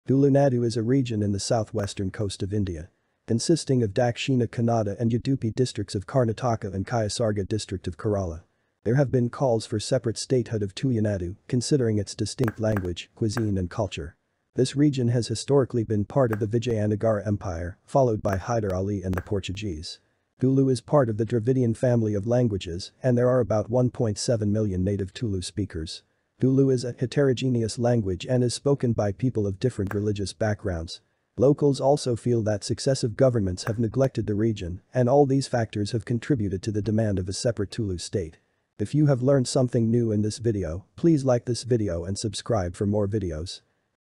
Tulunadu? | Tulu Language and demand for a separate state!!!
tulunadu is a region in the south-western coast of india, which has been demanding a state, separate from karnataka. In this video we have highlighted the factors that have contributed to the demand of a separate
tulu state